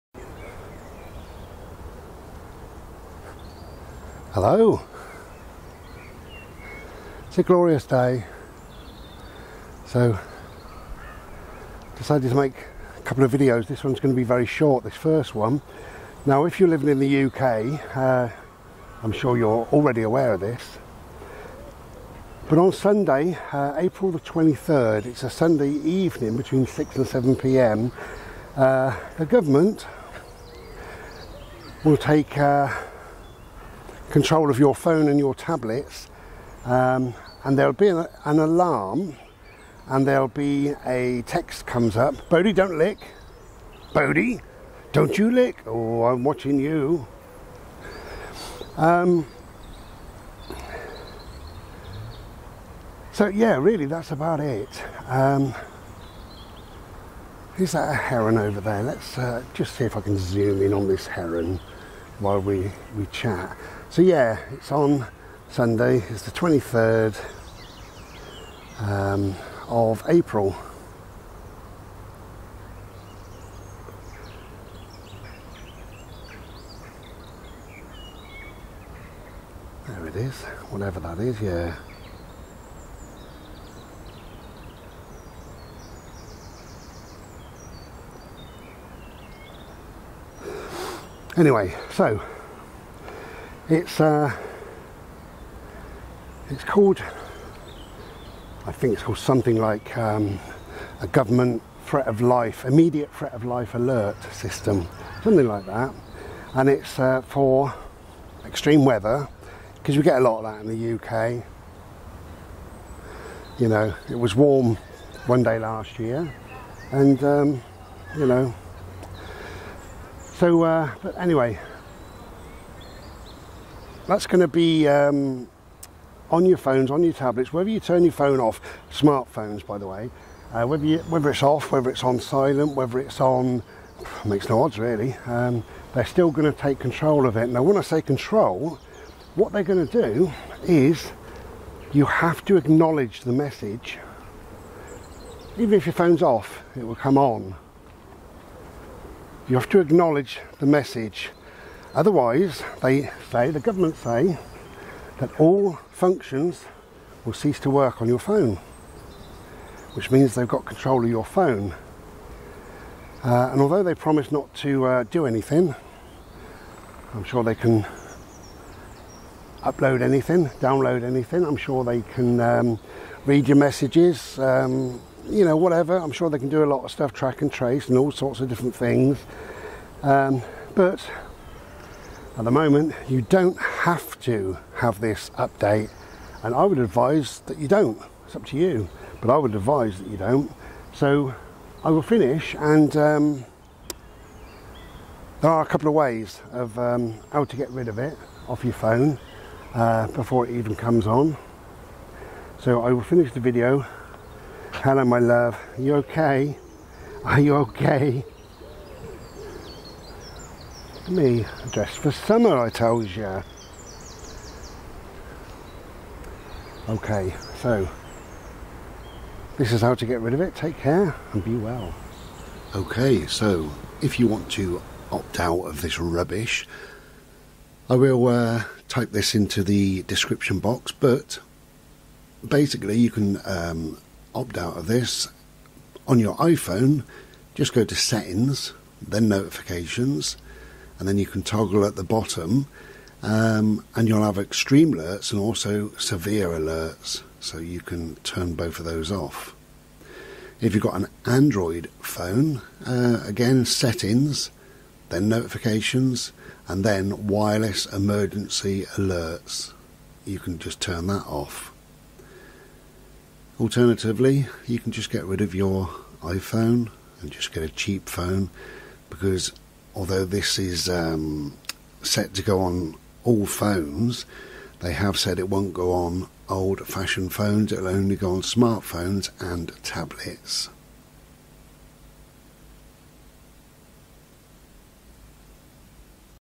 How to disable government alerts on your phone
You can turn off emergency alerts on your phone settings at any time.
On iPhone, this is under ‘Settings’, and then ‘Notifications’ – with toggles at the bottom to turn off ‘Extreme Alerts’ and ‘Severe Alerts’.
On Android, emergency alerts are found under ‘Settings’, then ‘Notifications’, then ‘Wireless Emergency Alerts’.

++Download AD BLOCKER, you'll never see another advert, plus it is FREE!!++